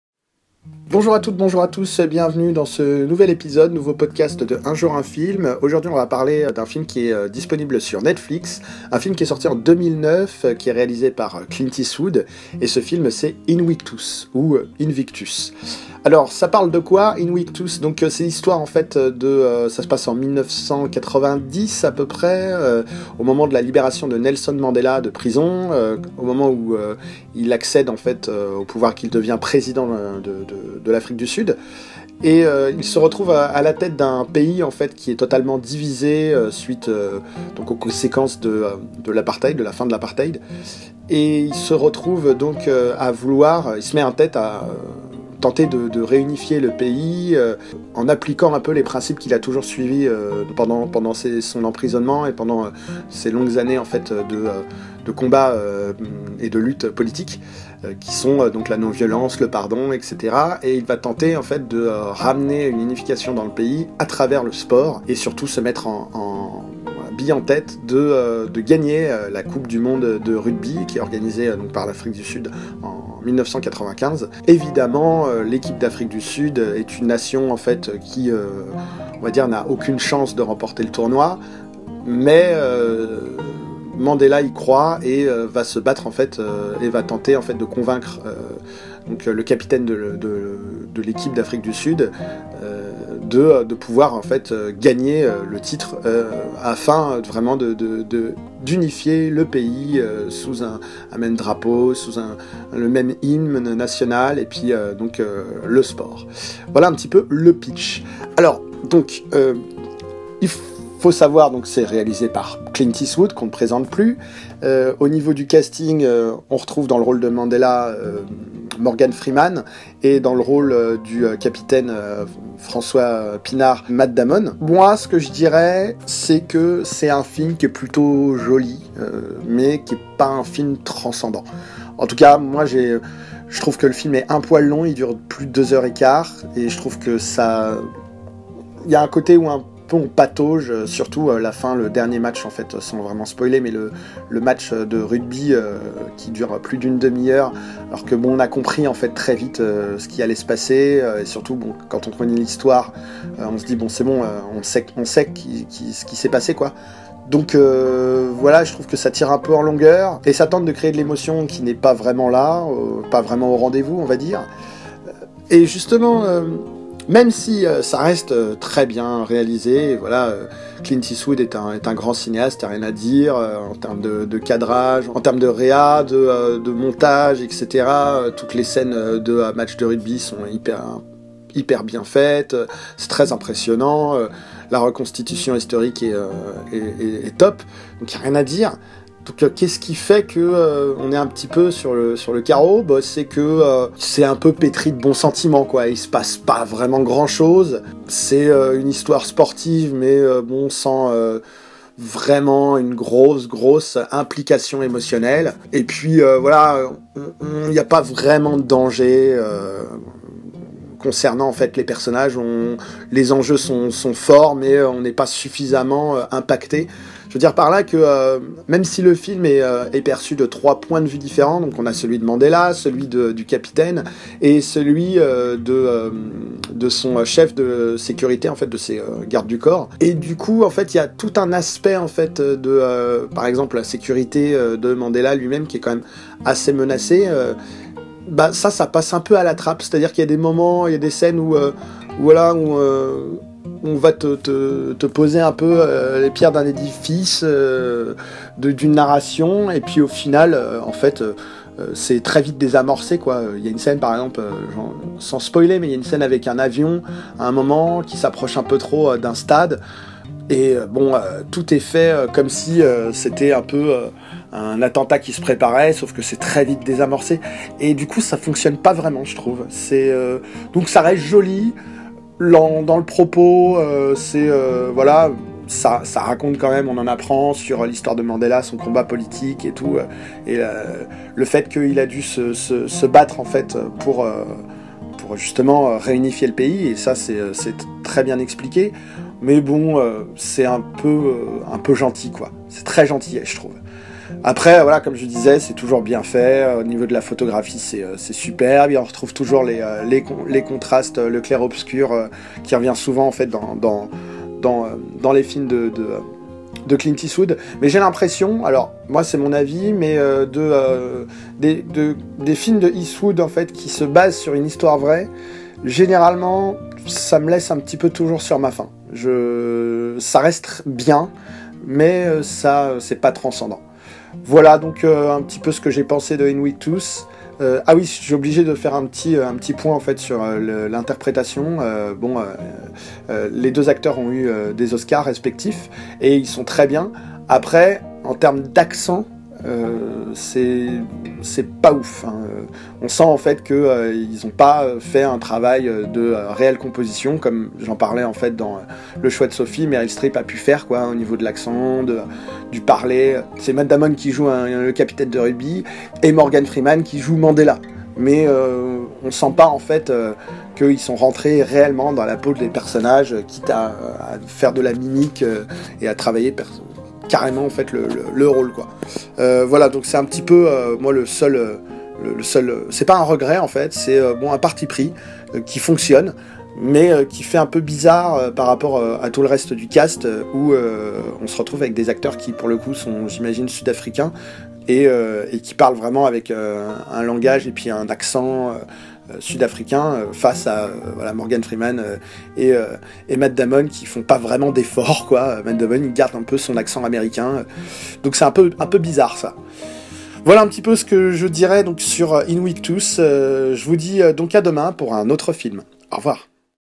UN JOUR, UN FILM - INVICTUS #41
Clint Eastwood raconte Mandela et la coupe du monde Rugby 1995. C'est bien fait, bien joué, mais c'est pas transcendant. Trop de bons sentiments peut être... Reste un beau sujet mais étant fan de Mandela j'en attendais bien plus.
Note : 6,5/10